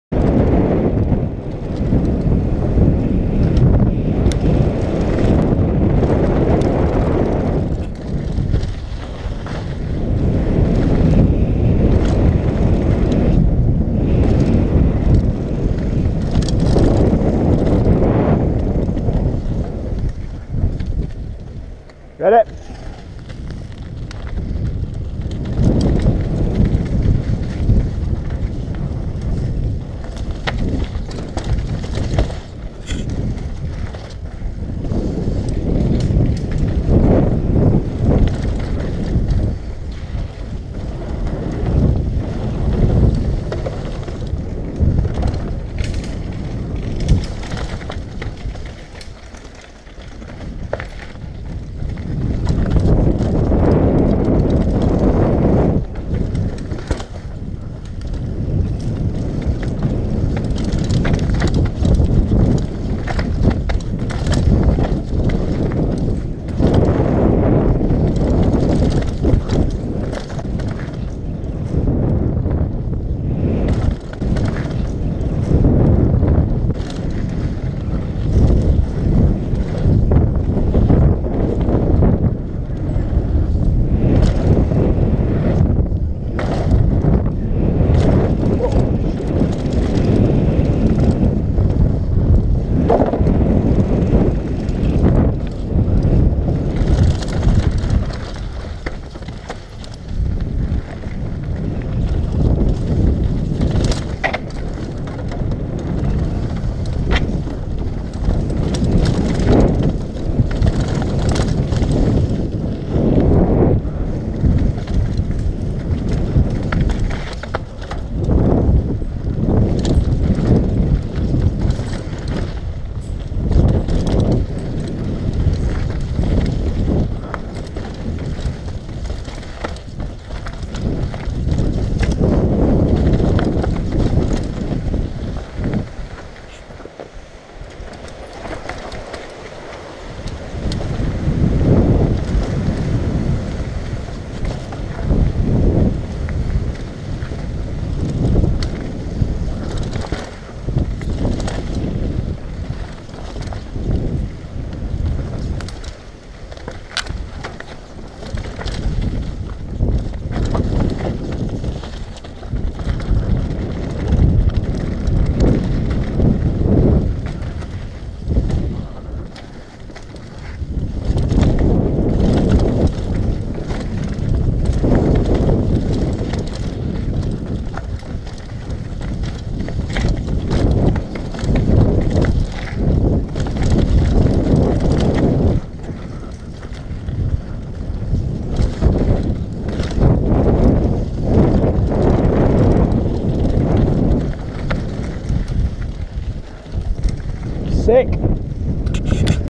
Fade to Black - Whistler Bike Park - Downhill MTB 2015
My first run of fade to black at Whistler bike park I'm riding a Santa Cruz Nomad carbon C with a clip of me doing the drop off on a Santa Cruz V10 from a different run. We start on the top of A-Line then into Ninja Cougar, Fade to Black, Karate Monkey and Samurai Pizza Cat. Filmed on GoPro Hero helmet side mount.